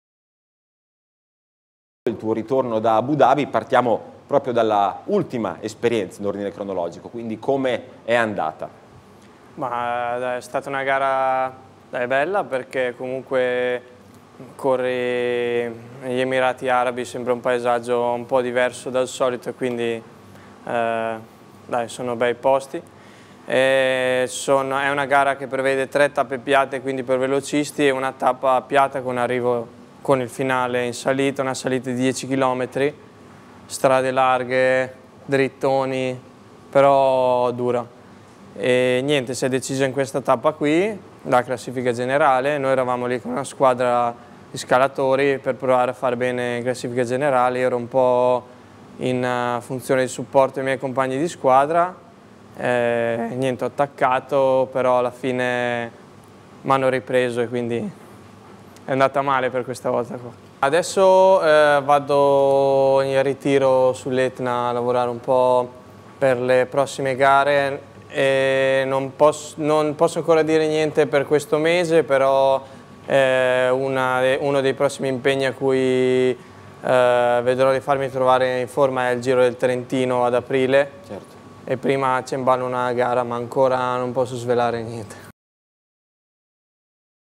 CICLISMO: La verità di Manuel Senni, "Soddisfatto a metà dell'inizio di stagione" | VIDEO
È tornato con qualche rimpianto Manuel Senni dall’Abu Dhabi Tour, una prova vinta dal portoghese Rui Costa: "È stata una gara bella, vista anche l'ambientazione molto suggestiva. Tutto si è deciso nel'ultima tappa: ho provato un attacco ma non sono riuscito a portarlo a termine e sono stato ripreso. Peccato". Il cesenate è comunque già concentrato sulla ripresa degli allenamenti in vista delle prossime corse: "Adesso vado in ritiro alle pendici dell'Etna per riprendere gli allenamenti in vista del prosieguo della stagione: parteciperò al Giro del Trentino ad aprile ma prima affronterò un'altra gara di cui non posso ancora svelare l'identità".